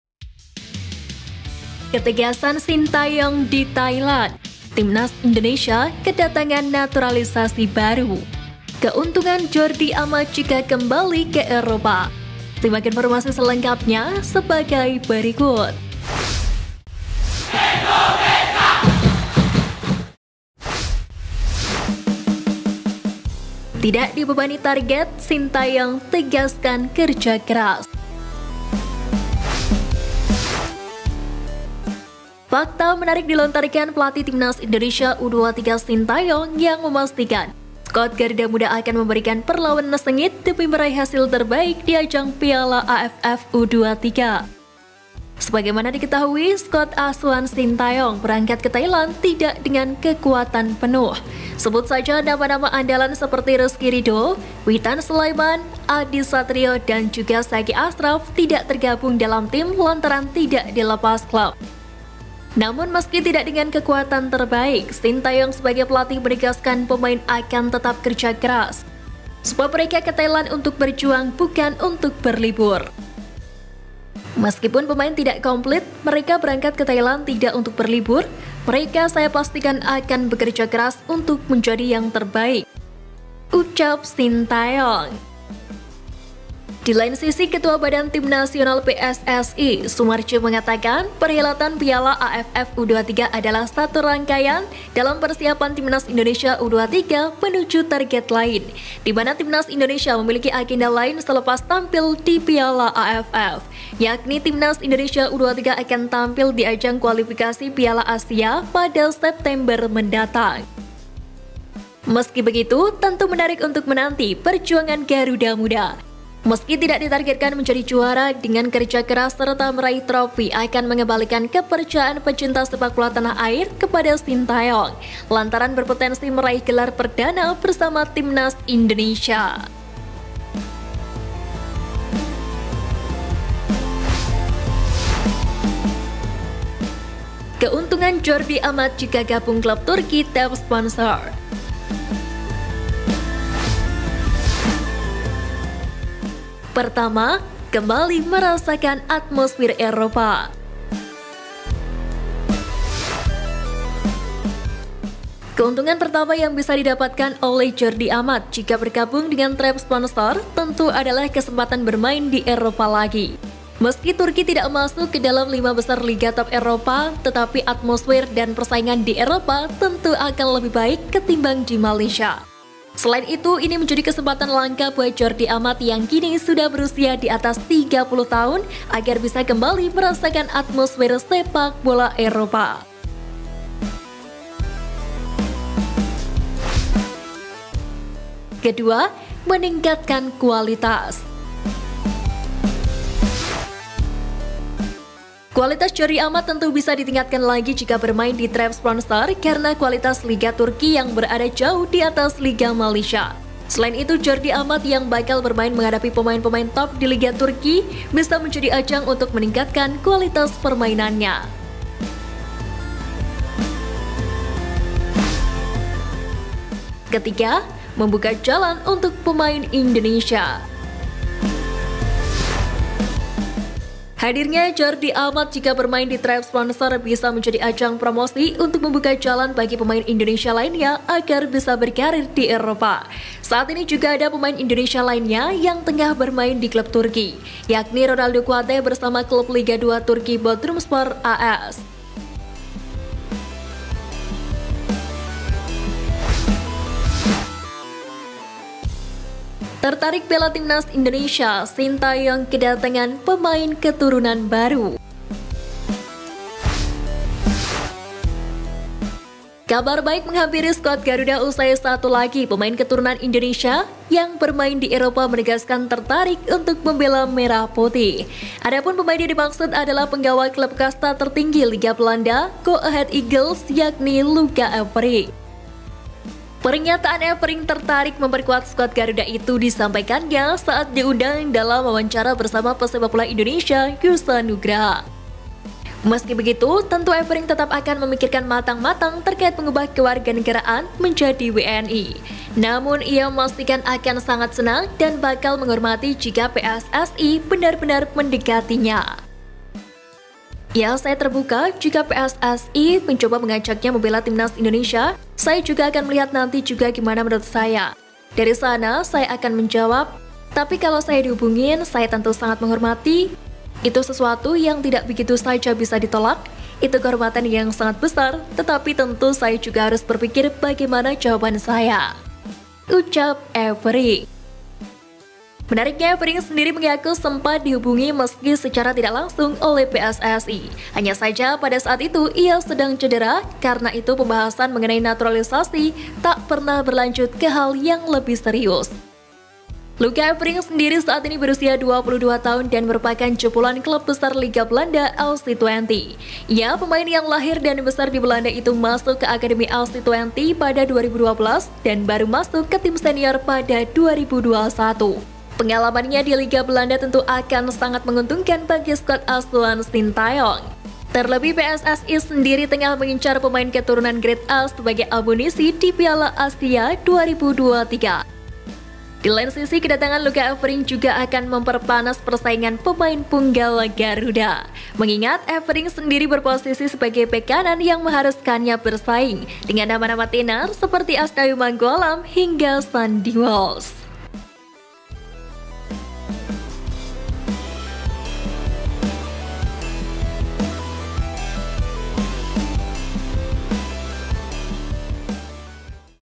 PENGGANTI RIZKY RIDHO?! Bek Belanda tertarik bela tanah kelahiran sang nenek~Timnas U23 siap tempur
BERITA BOLA TERBARU DAN TERUPDATE DARI GARUDA SPACE - Berita Bola Hari Selasa, 15 Agustus 2023 : PENGGANTI RIZKY RIDHO?! Bek Belanda tertarik bela tanah kelahiran sang nenek~Timnas U23 siap tempur